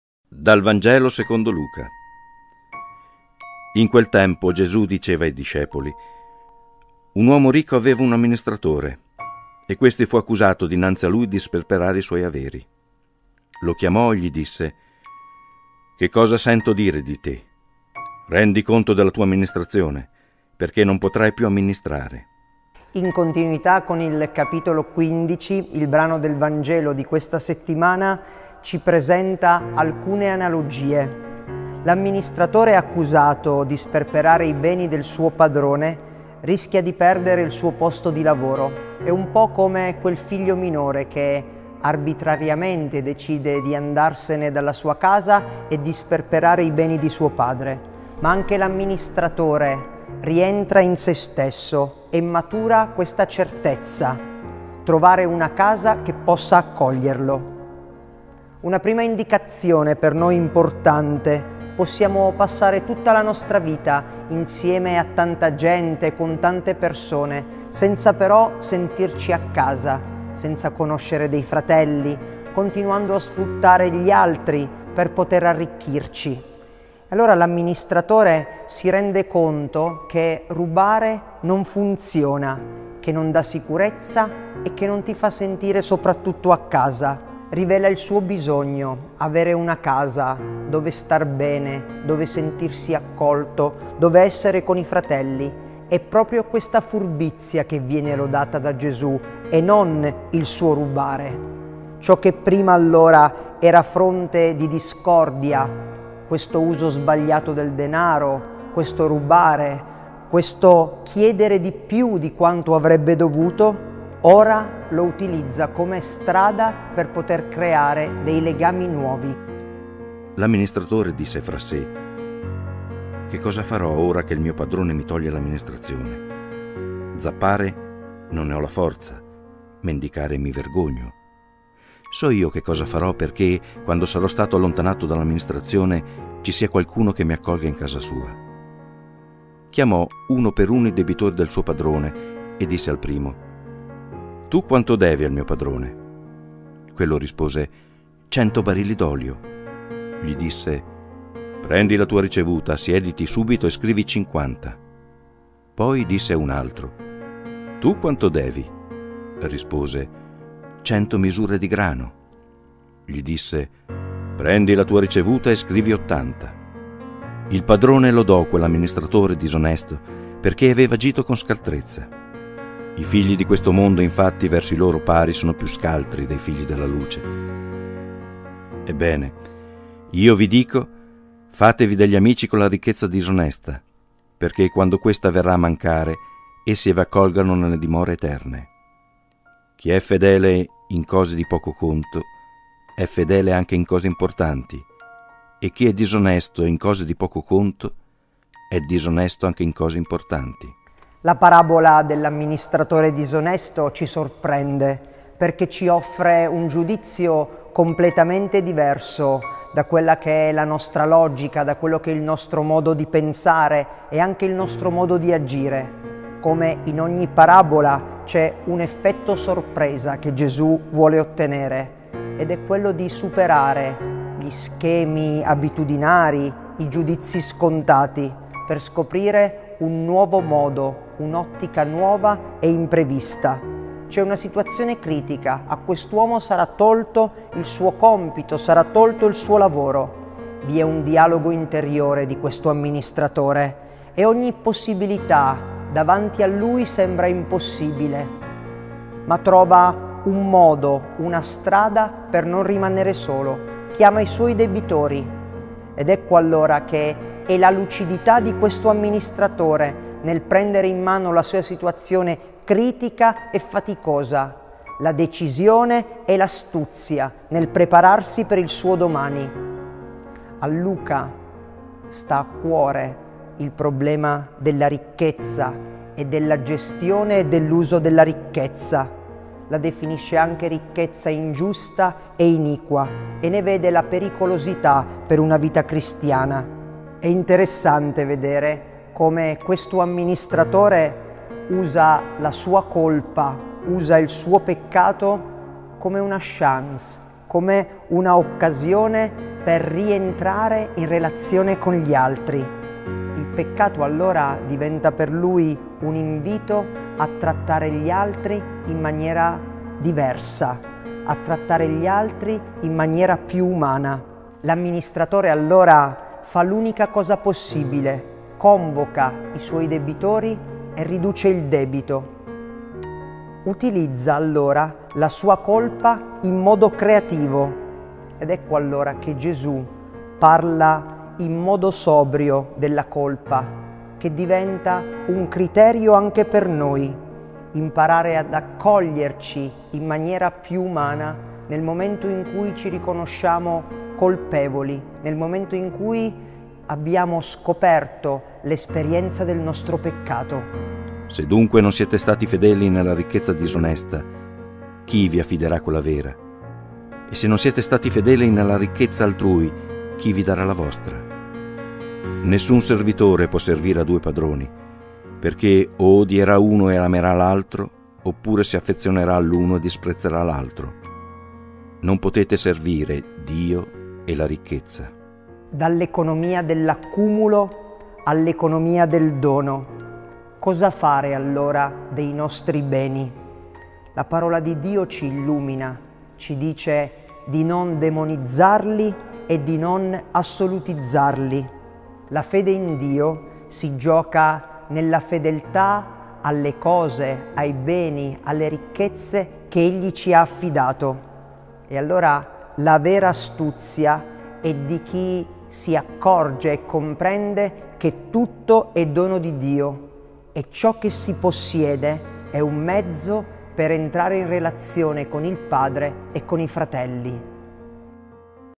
Non potete servire Dio e la ricchezza
Il Vangelo commentato da padre Domenico Fidanza